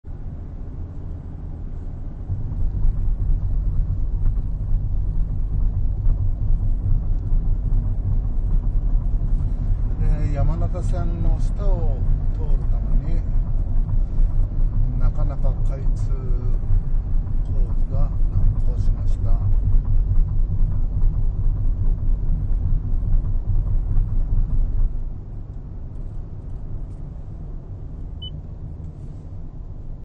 高田馬場駅バイパス完成60年
高田馬場駅近くのバイパスが60年以上かけてようやく完成しました。早稲田～小滝橋間の道路です。山手線や湘南新宿ライン・埼京線(昔は汽車が走っていました）や西武新宿線の下を抜けるトンネルが60年以上工事が進まなかったのですが開通しました。早稲田側出口 学校を欠席した子供向けに実験観察動画をつくっていますが授業の予習復習用などにも活用して下さい。